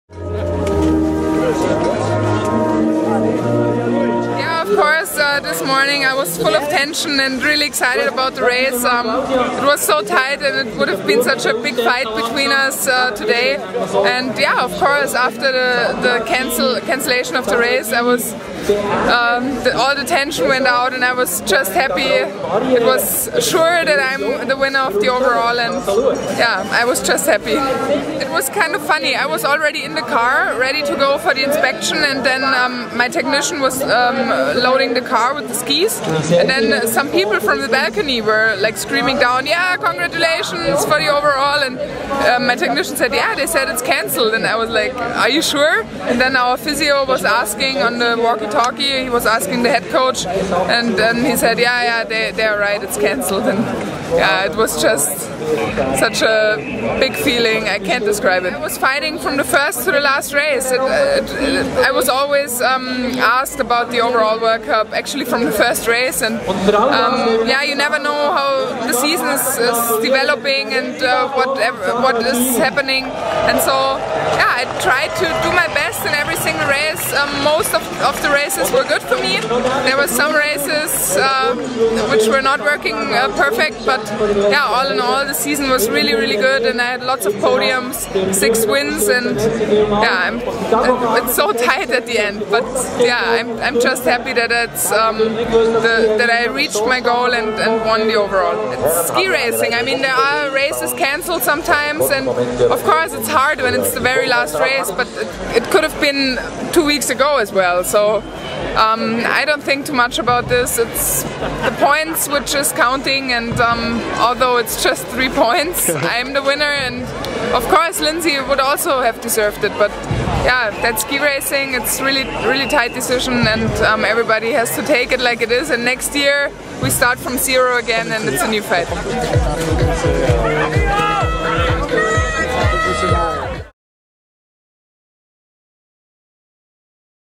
Maria Riesch: 2011 World Cup overall champion
The German took the big globe on 19 March 2011 in Lenzerheide, earning 1728 total points in the season, just three more than Lindsey Vonn, who took second.